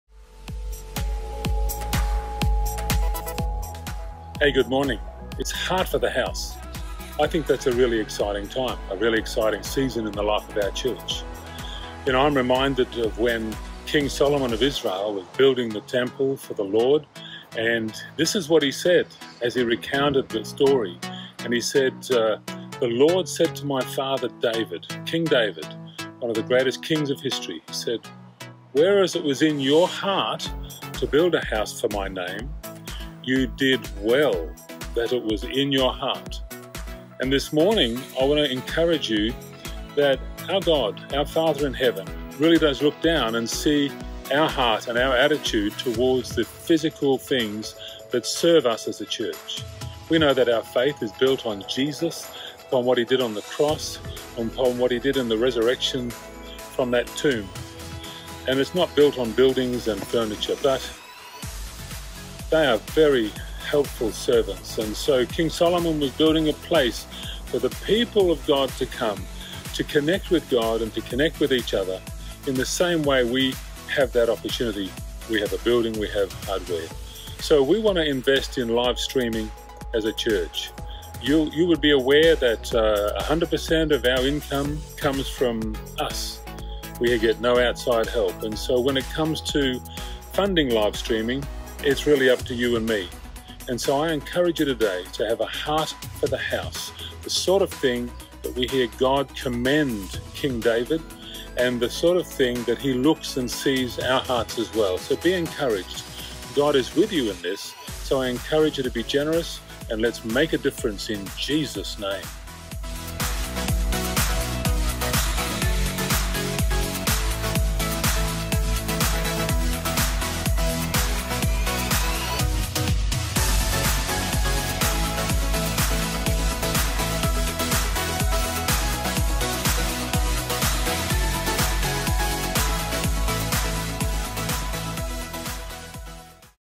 Heart for the house offering
During Yesterdays Service was our 'Heart for the house offering'

We are excited about our future, and what it holds for us!

Have a listen to Pastor Glenn sharing about why we are doing this and how you can contribute!